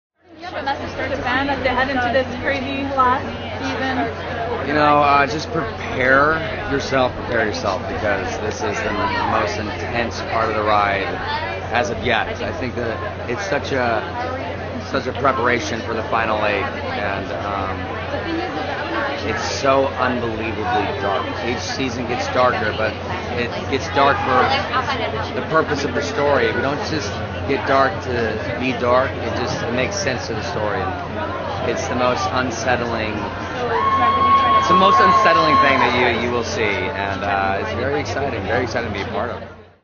Breaking Bad Season 5: Aaron Paul - Comic Con 2012
Here is Aaron Paul who plays Jesse Pinkman on the AMC Hit Series talking about the fifth and final season of the series.